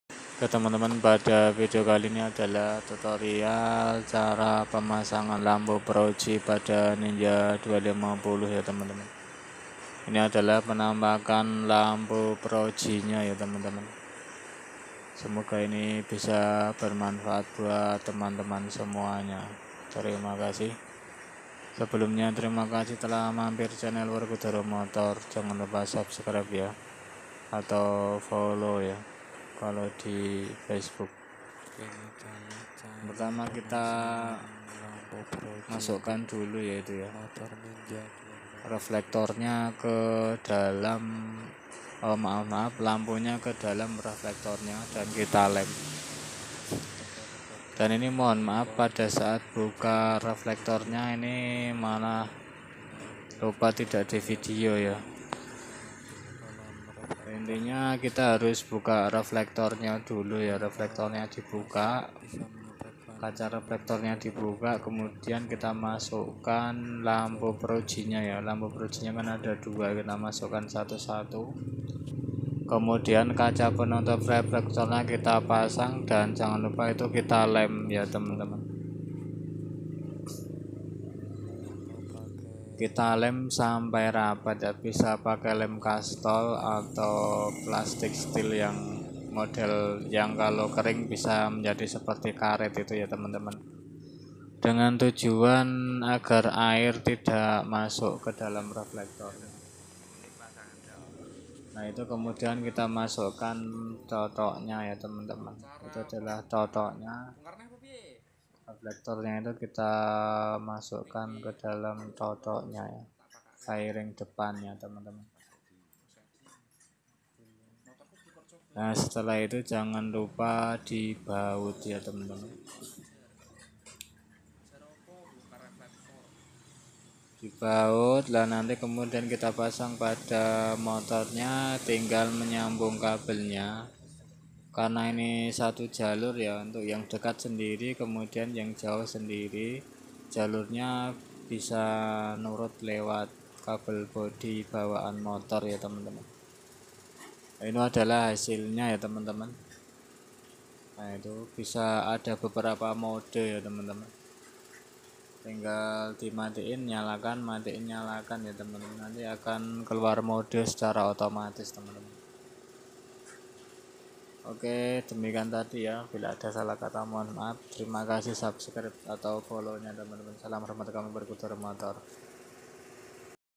Vidio cara pasang lampu pro ji ninja 250#68techwerkudoromotor
vidio tutorial cara pasang lampu pro ji ninja 250 semoga bermanfaat bagi anda semua dimanapun berada terimakasih salm hormat kami werkudoro motor di tunggu subscrib nya.